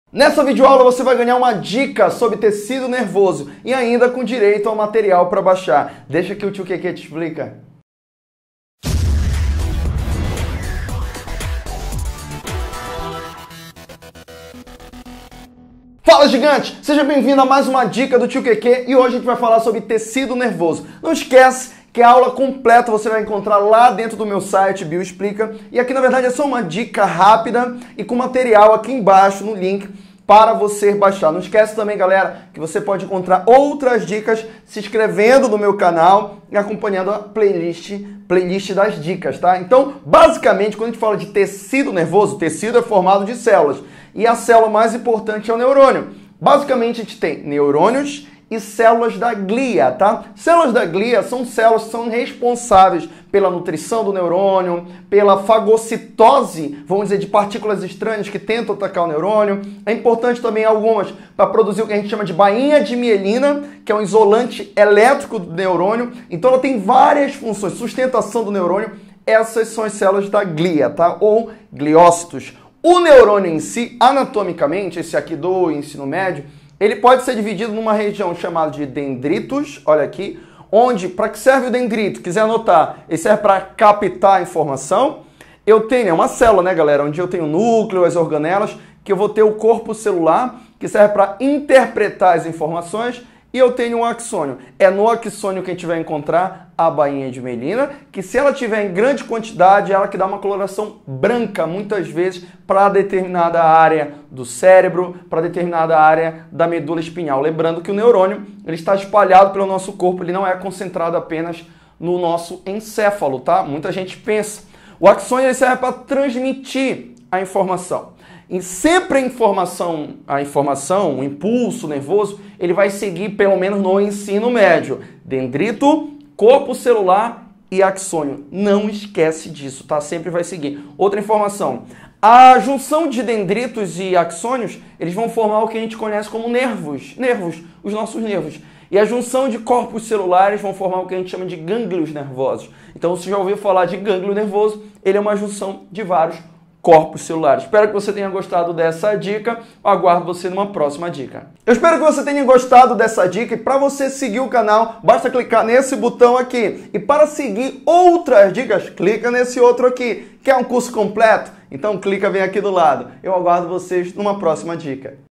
TECIDO NERVOSO - HISTOLOGIA - DICA ENEM BIOLOGIA #39 - Prof. Kennedy Ramos
Fala gingante, espero ajudar com essas DICAS PARA O ENEM. Para quem quiser baixar materiais e assistir aulas completas, entra no meu site e deixa que o Tio KEKE te explica.